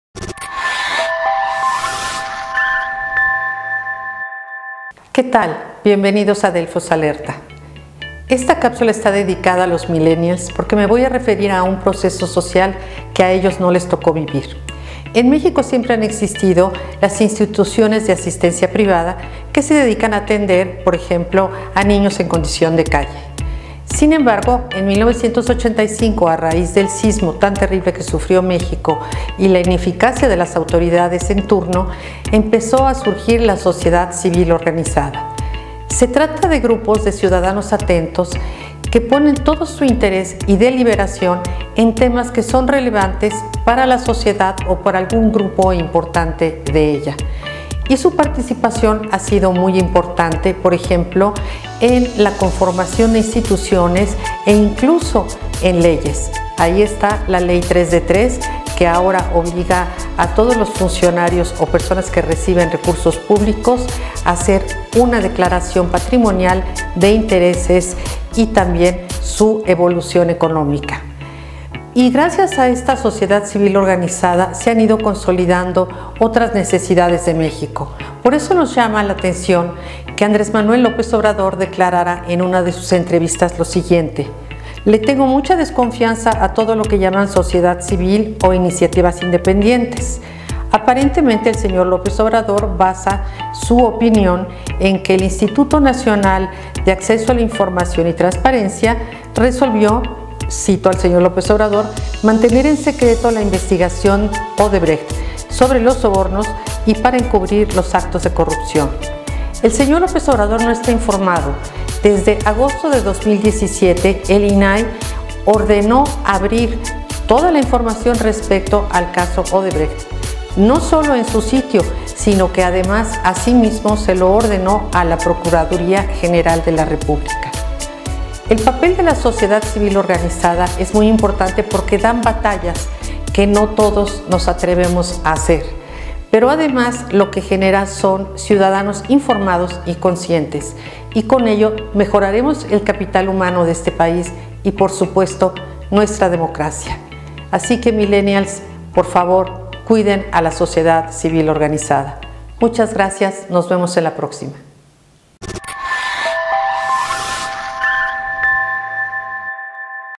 Delfos Alerta 11 ¿Qué es y en qué beneficia una Sociedad Civil Organizada
En México existen desde hace varios años instituciones de asistencia privada dedicadas a grupos de escasos recursos, a esas instituciones las llamamos Sociedad Civil Organizada que además de ayudar tienen un gran peso en la política de nuestro país.